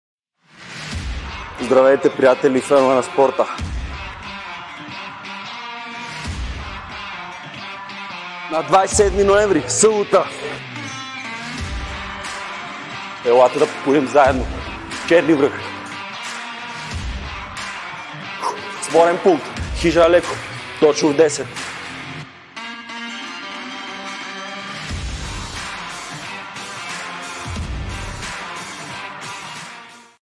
Покори Черни връх с Pulse Fitness & Spa ⛰️
✅ На 27.11 в 10:00 часа те очакваме пред хижа Алеко!
👉🏻 Включи се в организирания поход до най-високата точка на Витоша, воден от Георгиос Сърбинов!
🤩 Очакват те много изненади, награди и забавни игри със състезателен характер на върха!
👩🏼‍🤝‍🧑🏼 Всеки е добре дошъл!